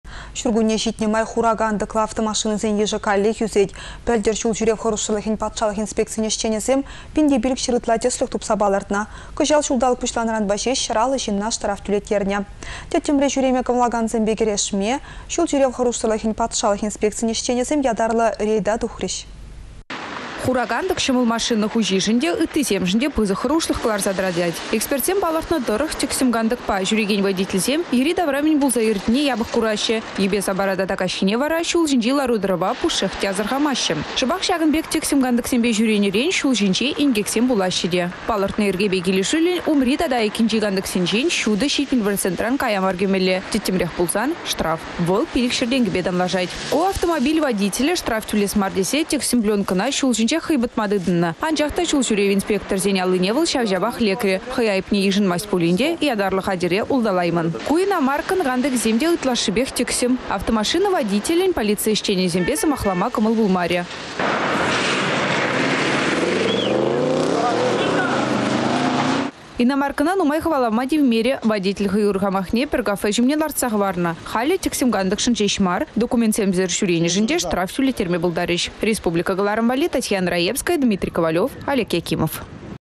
Тĕксĕм кантăкшăн штрафлаççĕ
Çуркунне çитнĕ май хура кантăклă автомашинăсен йышĕ Каллех ÿсет. Пĕлтĕр çул çÿрев хăрушсăрлăхĕн патшалăх инспекцийĕн ĕçченĕсем 1500 ытла тĕслĕх тупса палăртнă. Кăçал çулталăк пуçланнăранпа çеç 150 çынна штраф тÿлеттернĕ. Тĕттĕмре çÿреме кăмăллакансемпе кĕрешме, çул çÿрев хăрушсăрлăхĕн патшалăх инспекцийĕн ĕçченĕсем ятарлă рейда тухрĕç.